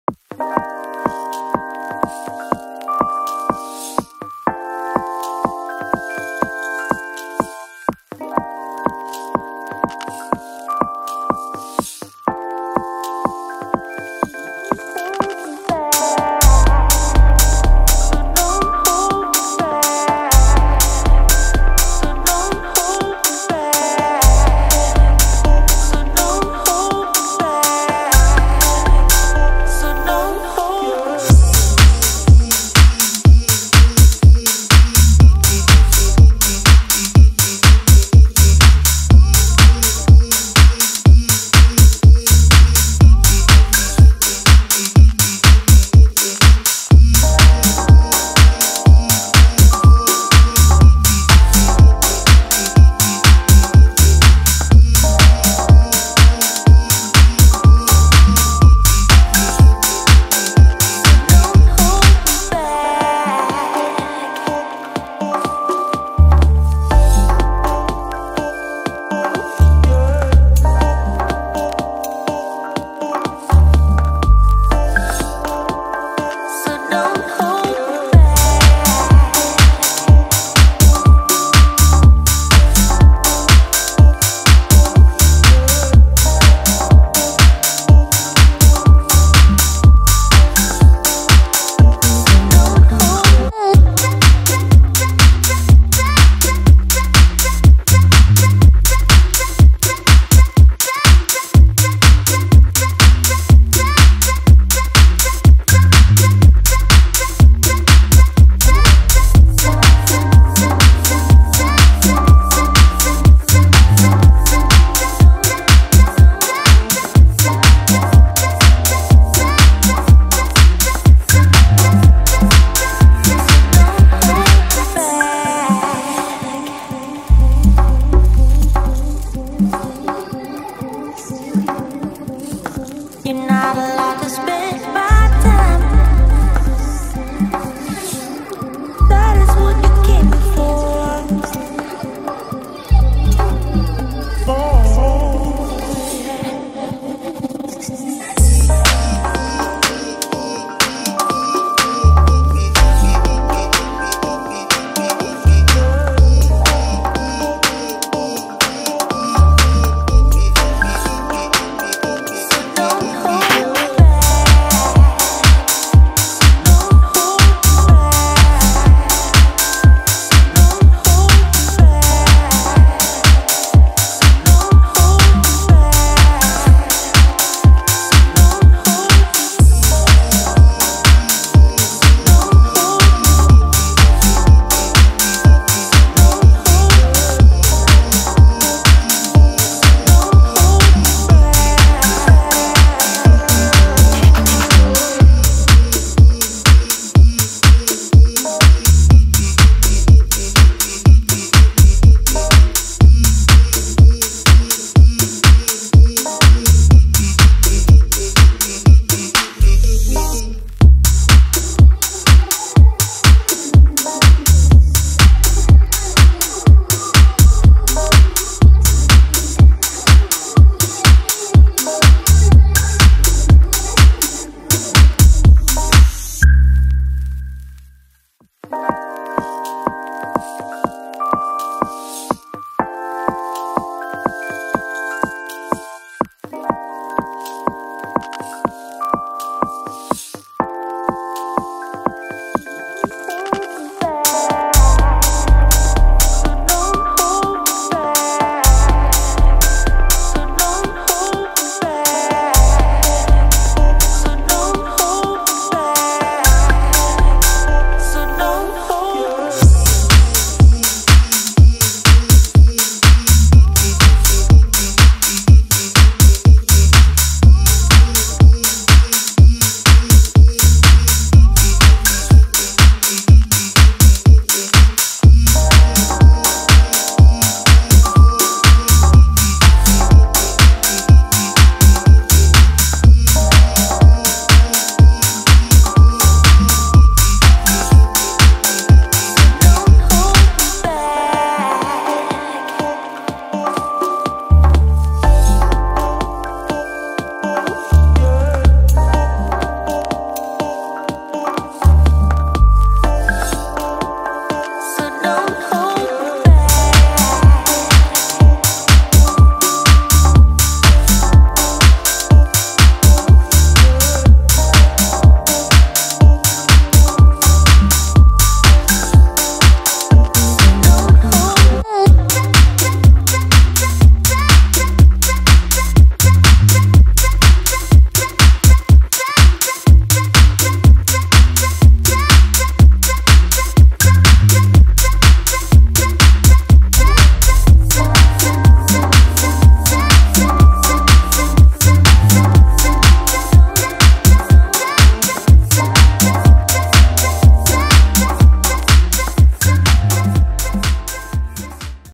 Waltham, MA (outside Boston) 2018
This video was shot at the Westin Waltham and around Rocket Software's HQ.